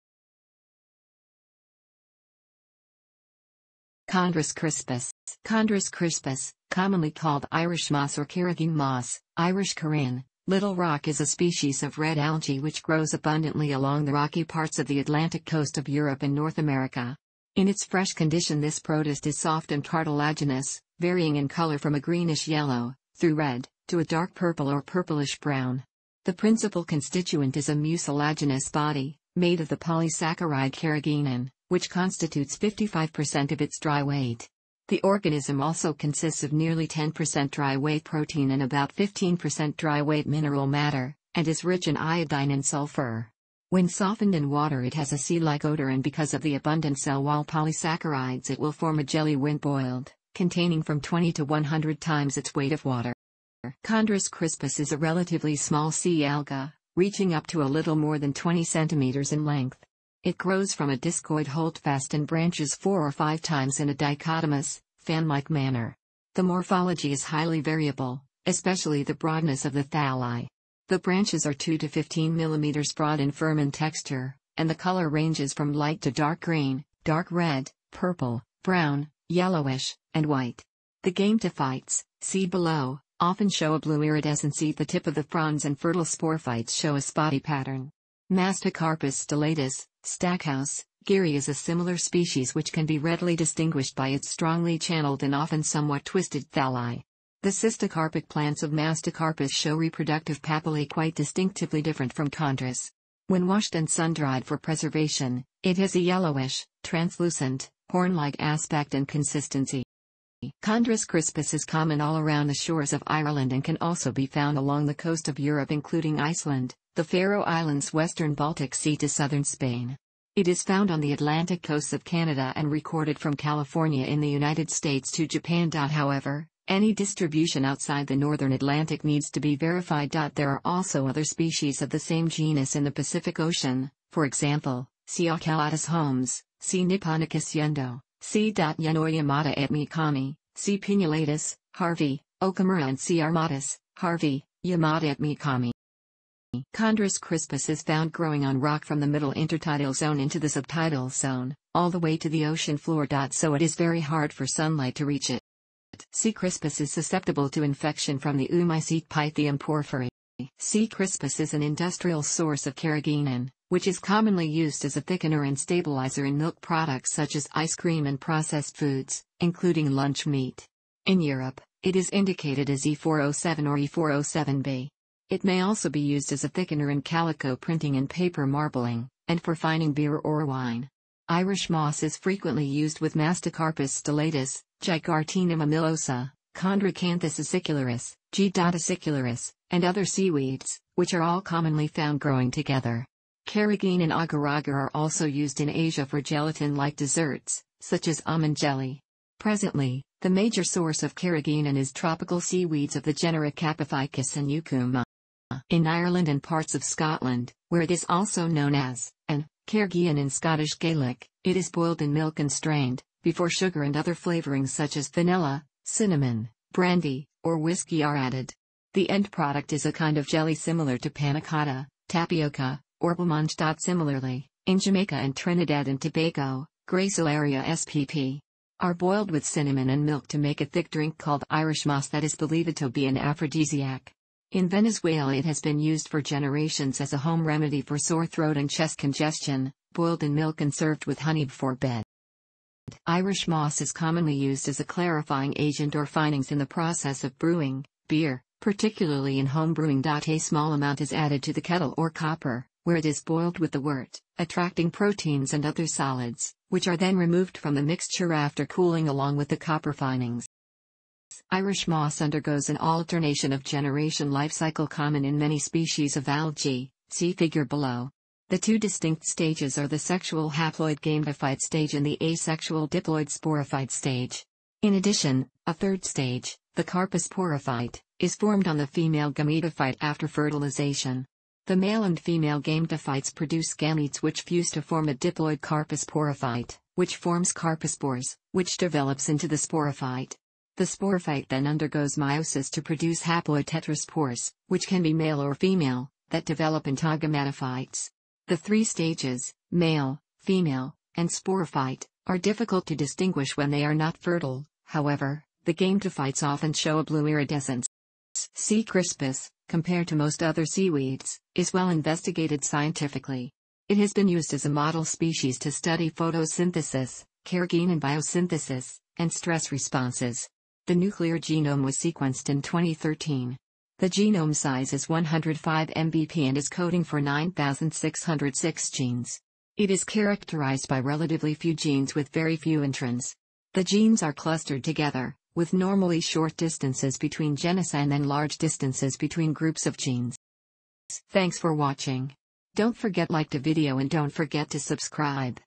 Chondrus crispus
Chondrus crispus—commonly called Irish moss or carrageen moss (Irish "carraigín", "little rock")—is a species of red algae which grows abundantly along the rocky parts of the Atlantic coast of Europe and North America. In its fresh condition this protist is soft and cartilaginous, varying in color from a greenish-yellow, through red, to a dark purple or purplish-brown. The principal constituent is a mucilaginous body, made of the polysaccharide carrageenan, which constitutes 55% of its dry weight. The organism also consists of nearly 10% dry weight protein and about 15% dry weight mineral matter, and is rich in iodine and sulfur. When softened in water it has a sea-like odour and because of the abundant cell wall polysaccharides it will form a jelly when boiled, containing from 20 to 100 times its weight of water.

"Chondrus crispus" is a relatively small sea alga, reaching up to a little more than 20 cm in length. It grows from a discoid holdfast and branches four or five times in a dichotomous, fan-like manner. The morphology is highly variable, especially the broadness of the thalli. The branches are 2–15 mm broad and firm in texture, and the color ranges from light to dark green, dark red, purple, brown, yellowish, and white. The gametophytes (see below) often show a blue iridescence at the tip of the fronds and fertile sporophytes show a spotty pattern. "Mastocarpus stellatus" (Stackhouse) Guiry is a similar species which can be readily distinguished by its strongly channelled and often somewhat twisted thalli. The cystocarpic plants of "Mastocarpus" show reproductive papillae quite distinctively different from "Chondrus". When washed and sun-dried for preservation, it has a yellowish, translucent, horn-like aspect and consistency.

"Chondrus crispus" is common all around the shores of Ireland and can also be found along the coast of Europe including Icela...